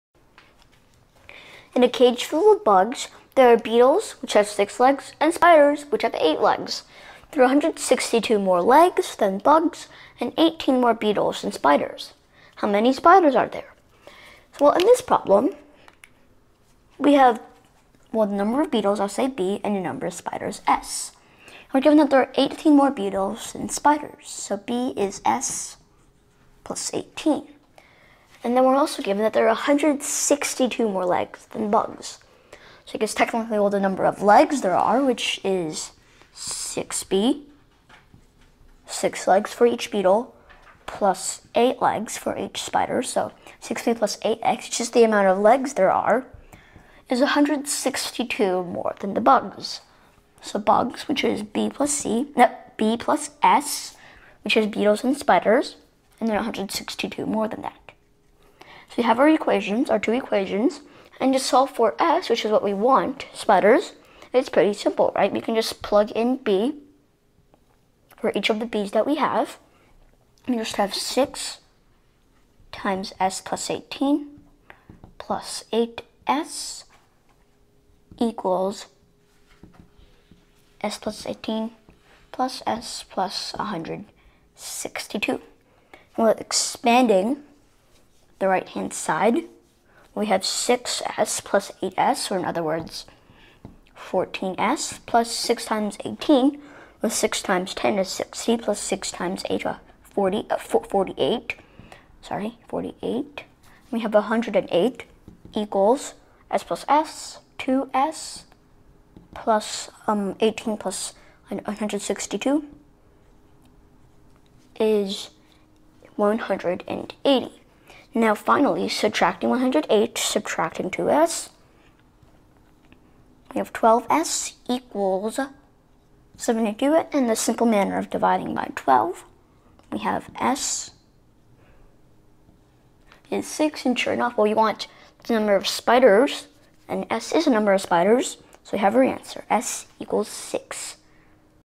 How many spiders are there? (See details in the description)
In a cage full of bugs, there are beetles (6 legs) and spiders (8 legs). There are 162 more legs than bugs and 18 more beetles than spiders. How many spiders are there?